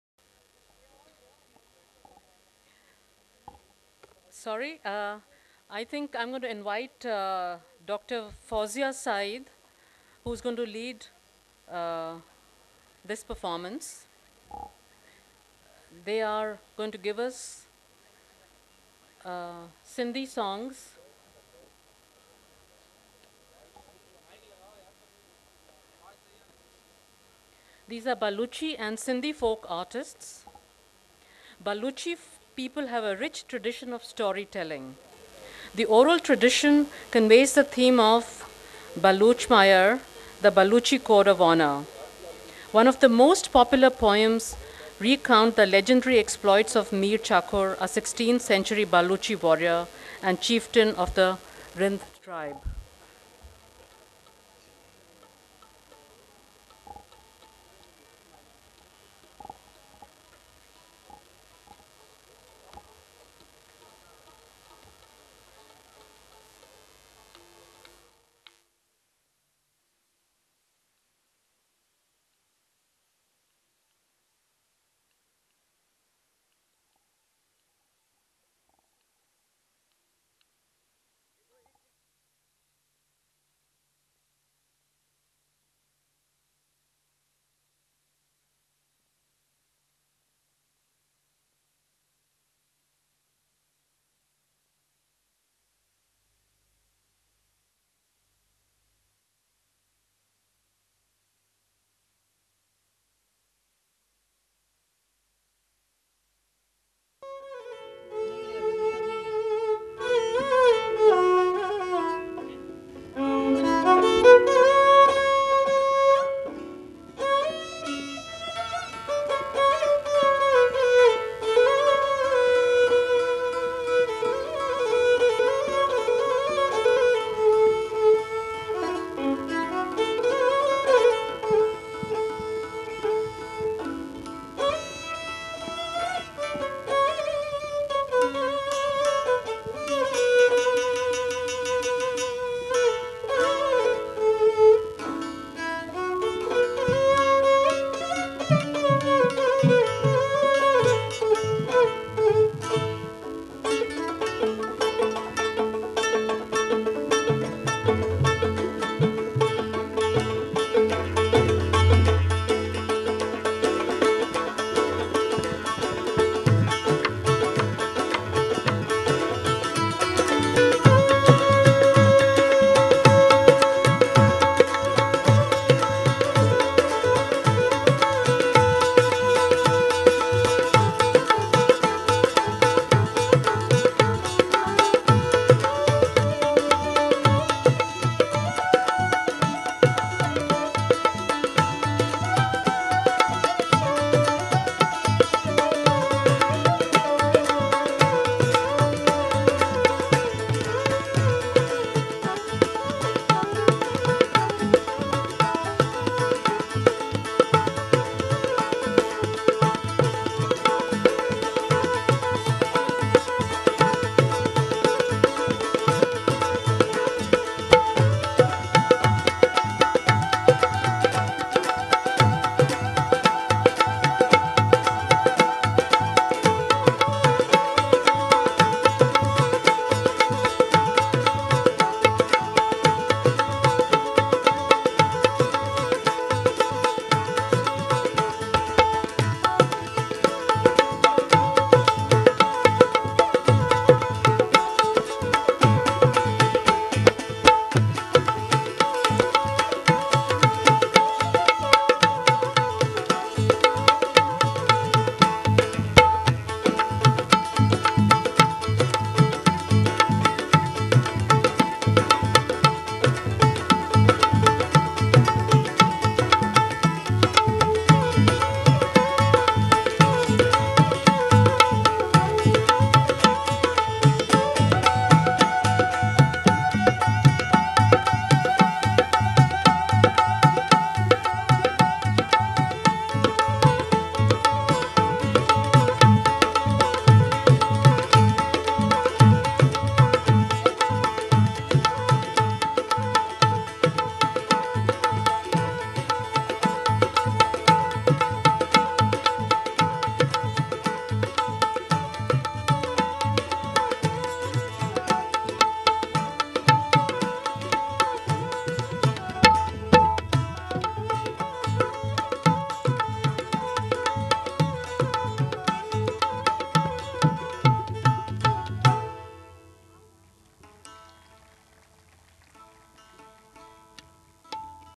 SAARC FOLKLORE FESTIVAL - New Delhi, December 5, 6, 7, 8, 9, 2007 -5
SAARC FOLKLORE FESTIVAL - New Delhi, December 5, 6, 7, 8, 9, 2007

Foundation of SAARC Writers and Literature, in collaboration with the Indian Council for Cultural Relations, Ministry of External Affairs; Ministry of Culture; the Central Institute of Indian Languages, Mysore; Sangeet Natak Akademi; and the cultural organisations and Governments of the SAARC countries, organized the first-ever SAARC Folklore Festival on December 5, 6, 7, 8 and 9, 2007.